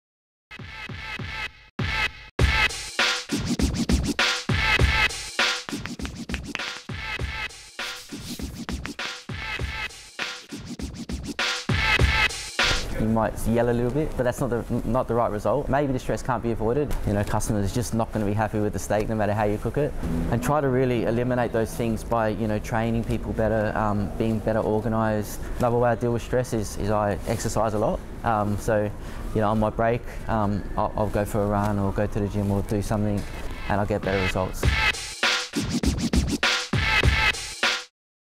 Daniel Hughes on dealing with stress in the kitchen
Daniel Hughes of Manta Woolloomooloo says that stress is unavoidable - your customer may never like the way you make his or her steak. And he shares his tips on what to do to take away the stress. Well, what would you as a Chef do? Tell us in comments.